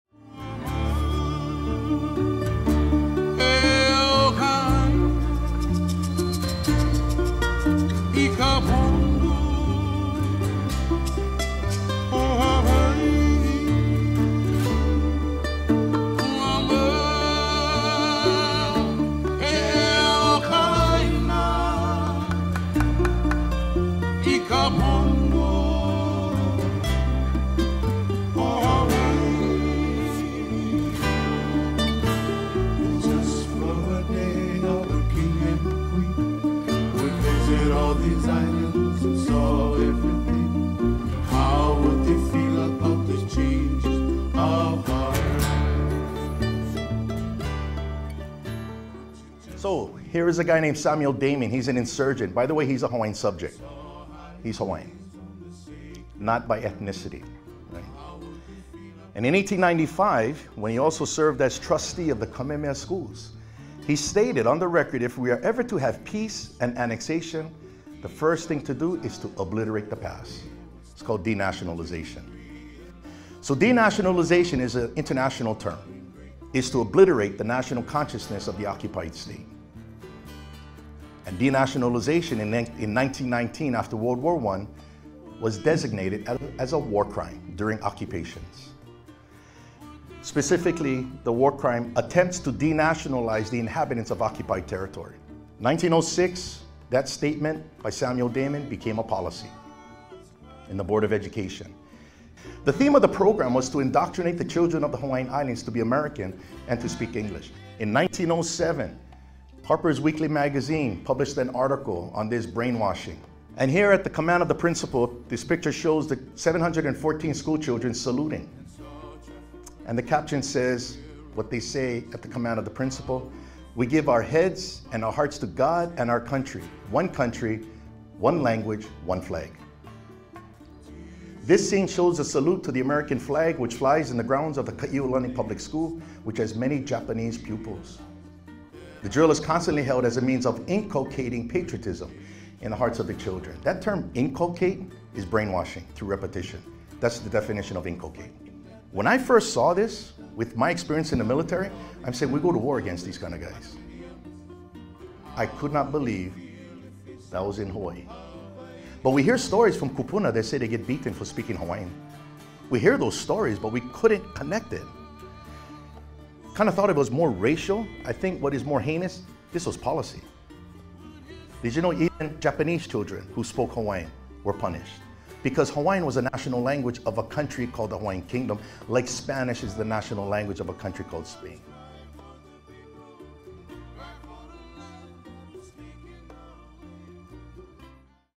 The Hawaiian Kingdom: Denationalization and Brainwashing
Dr. Keanu Sai presents to the Maui County Council on how the United States enforced denationalization, a war crime, onto the people of the Hawaiian Kingdom.